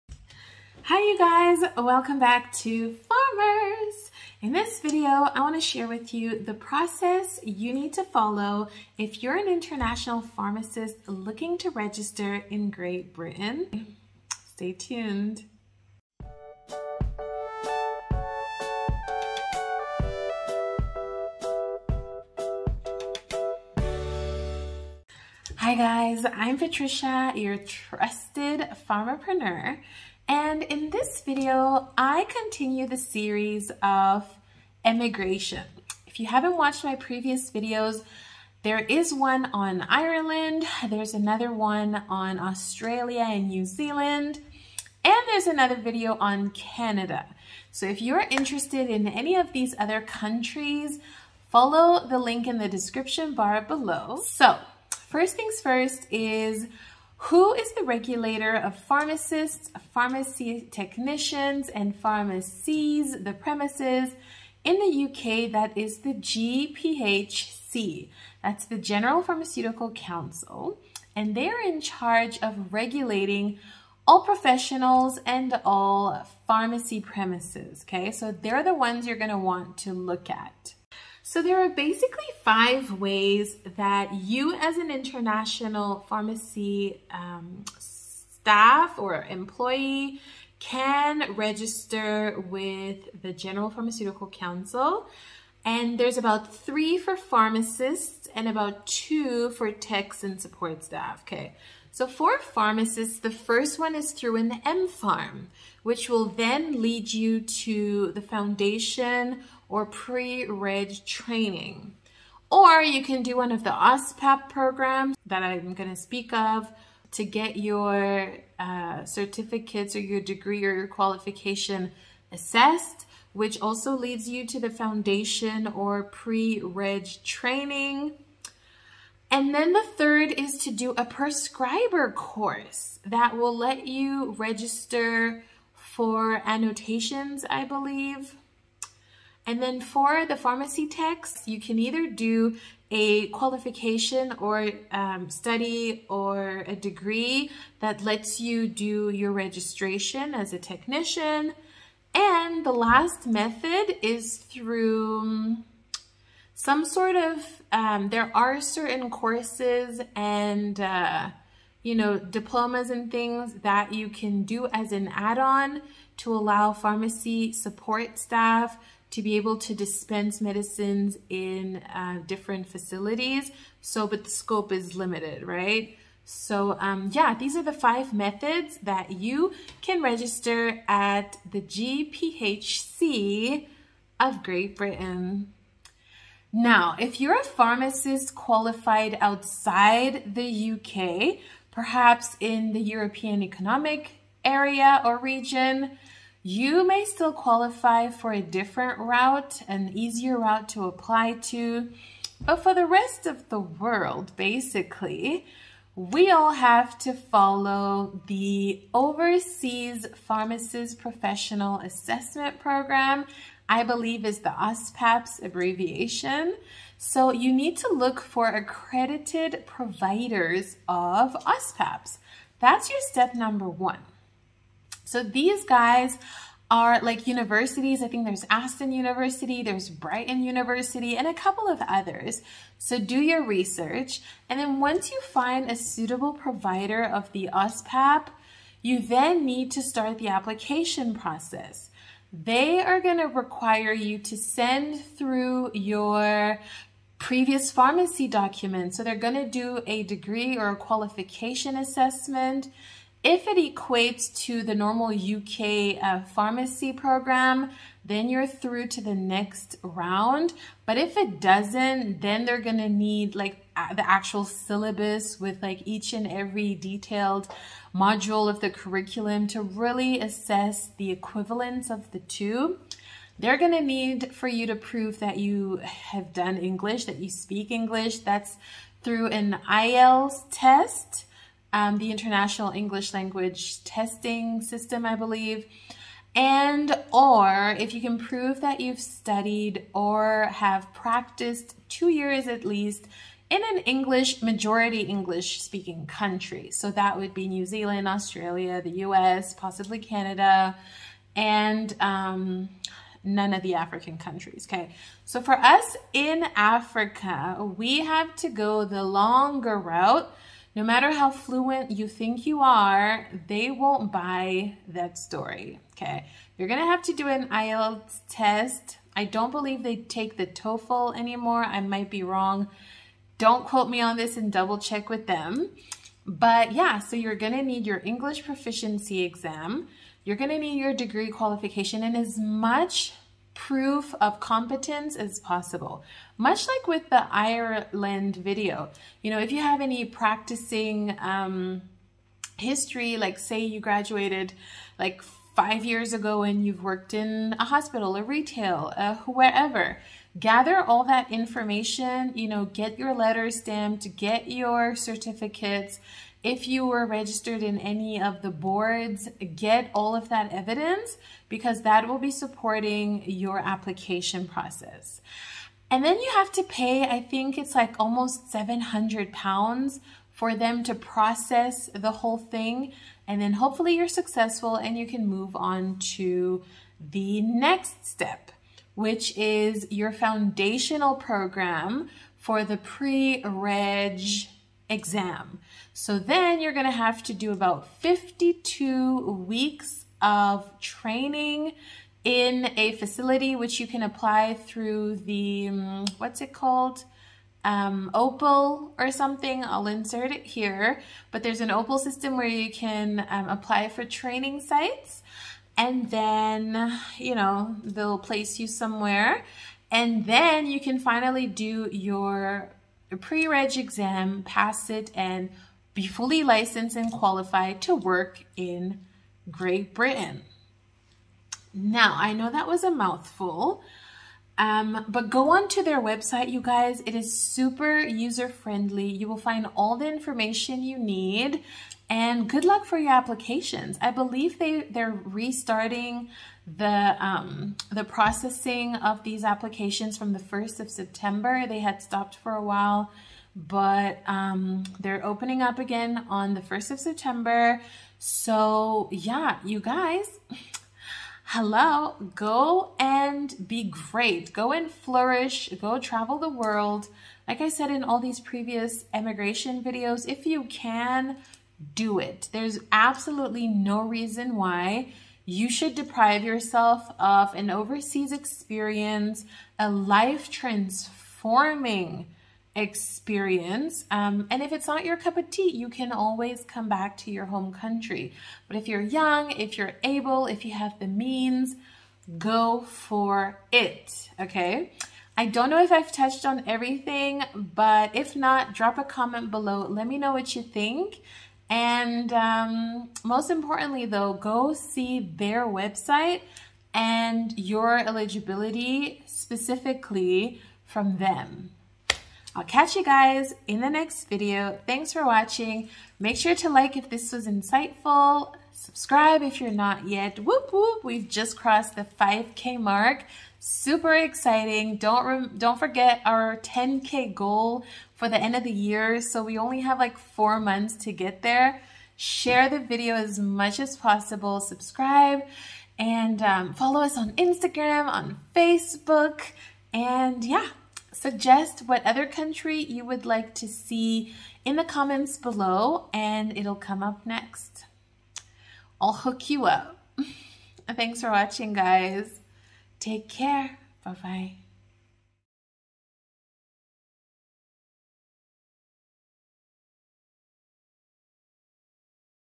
How To Register & Work As A Foreign Pharmacist/ Tech in Great Britain 🇬🇧 | 2022 Costs
The emigration series continues with Great Britain.

Timestamps:
GPhc 1:00
3 methods 1:35
OSPAP 3:11
52 weeks training & Oriel system 6:20
Further info 7:02
Costs 6:07